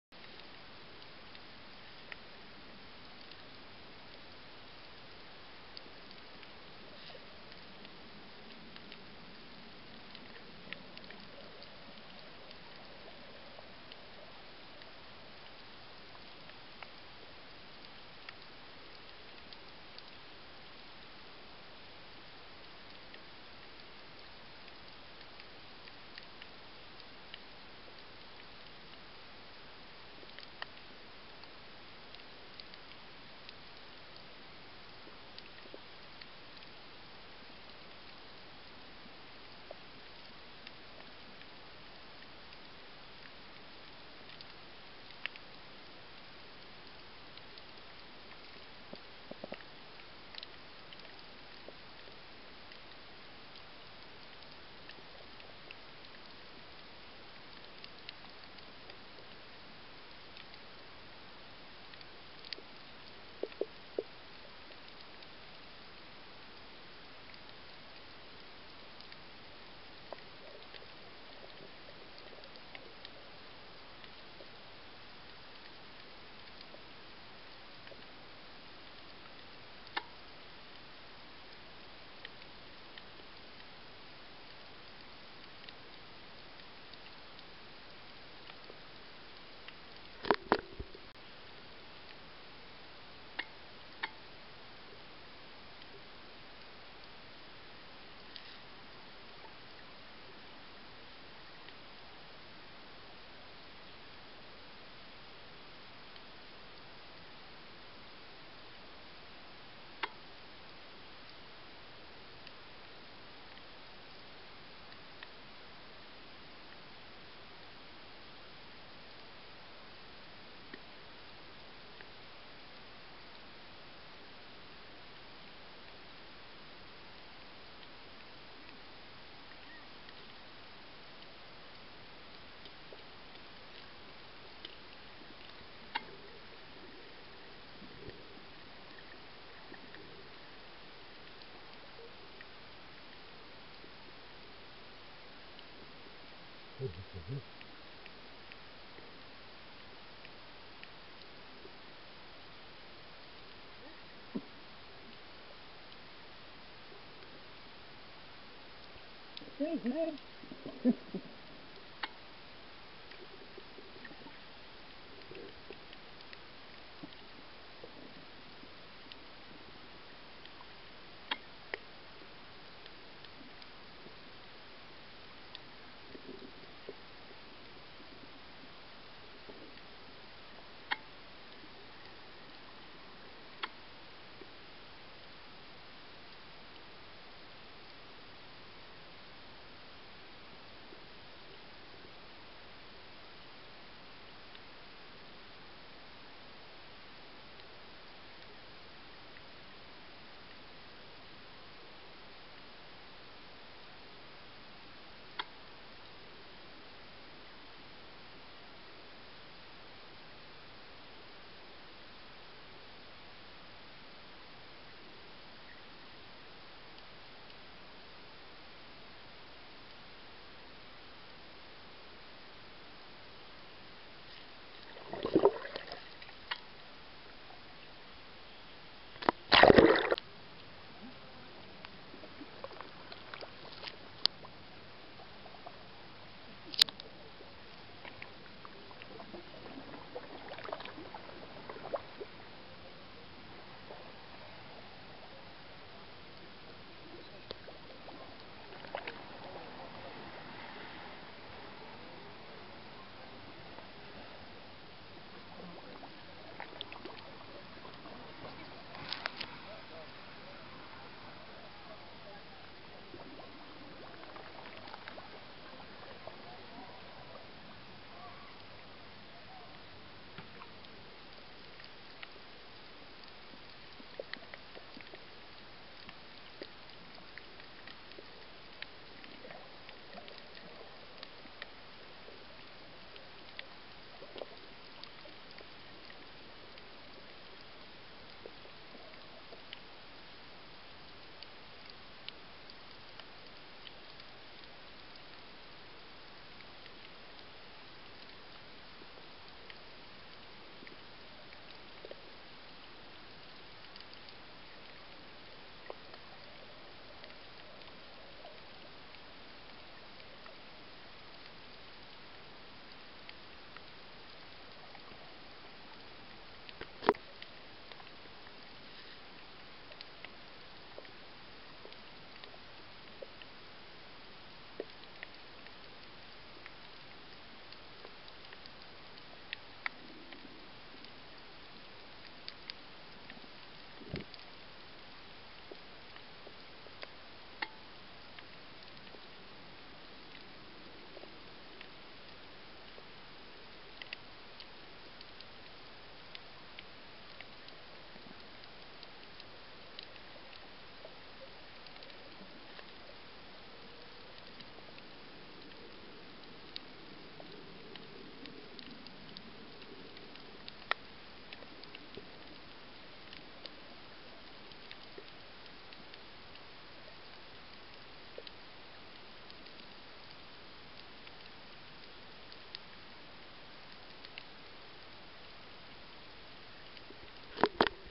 Snorkelling with the Stingrays!.wmv
About 40 minutes prior to the Stingrays being fed at Chaaya Reef, Ellaidhoo, Maldives we snorkelled in the area and found that they were hanging around in preparation for their supper.  The large fish in the video are enormous which makes the prospective difficult to compare.  All I know is that the fish are of the mackerel family.  Needless to say, we got out before they started chucking the food in as we didn't want to be caught up in the frenzy LOL